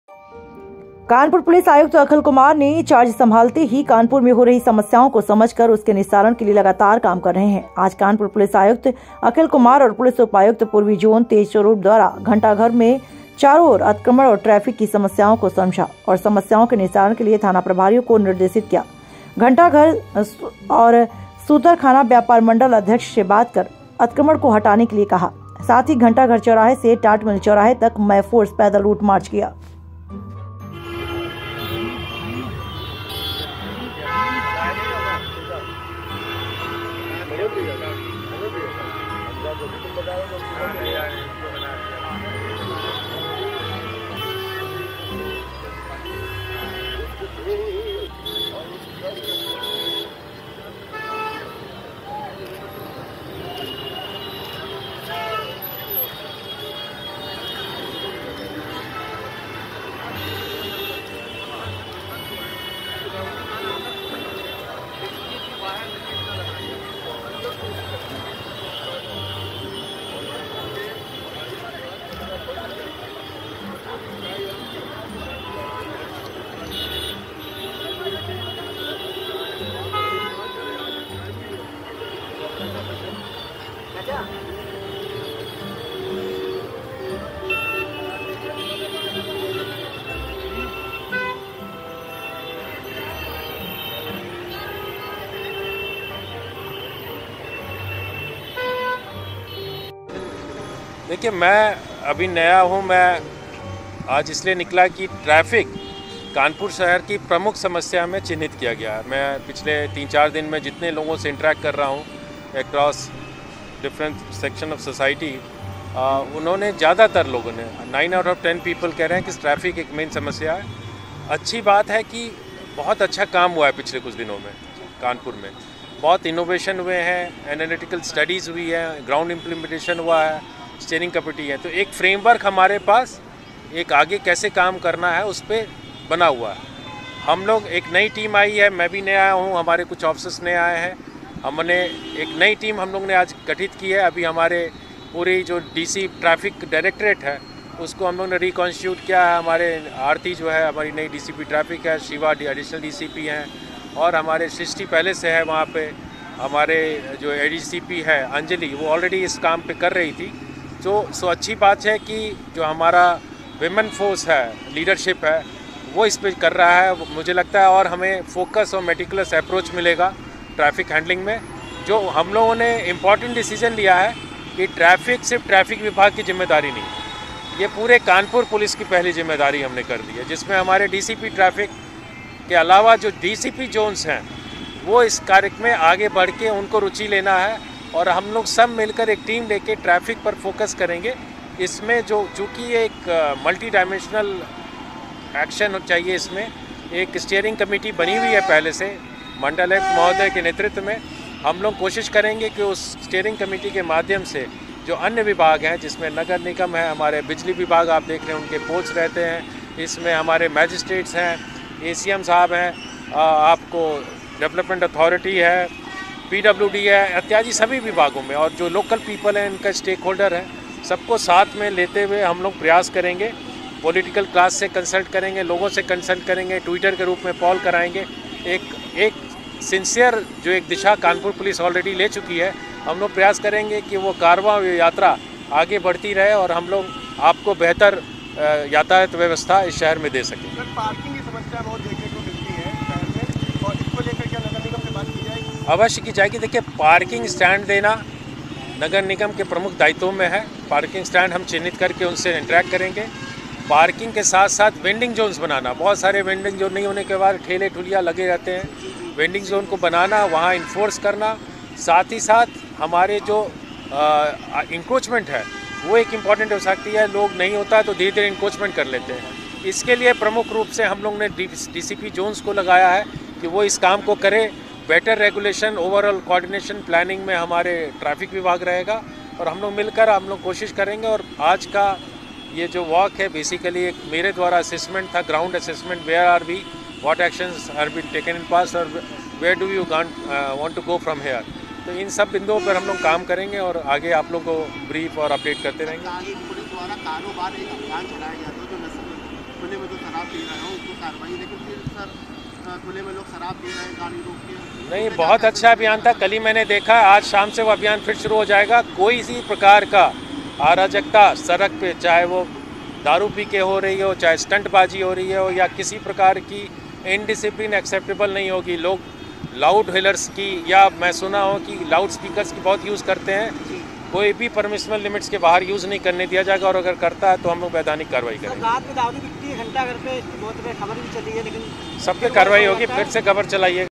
पुलिस आयुक्त अखिल कुमार ने चार्ज संभालते ही कानपुर मे हो रही समस्याओं को समझ कर उसके निस्तारण
संवाददाता सुजीत कुमार

कानपुर :- पुलिस आयुक्त अखिल कुमार ने चार्ज संभालते ही कानपुर मे हो रही समस्याओं को समझ कर उसके निस्तारण के लिए लगातार काम कर रहे है। आज कानपुर पुलिस आयुक्त अखिल कुमार और पुलिस उपायुक्त पूर्वी जोन तेज स्वरुप द्वारा घंटाघर के चारो और अतिक्रमण और ट्रैफिक कि समस्याओं को समझा और समस्याओं के निस्तारण के लिए थाना प्रभारियों को निर्देशित किया गया और घंटाघर सुतरखाना व्यापार मंडल अध्यक्ष से बात कर अतिक्रमण हटाने को कहा। साथ ही घंटाघर चौराहे से टाटमिल चौराहे तक मय फोर्स पैदल रूट मार्च किया।

बाइट///पुलिस आयुक्त अखिल कुमार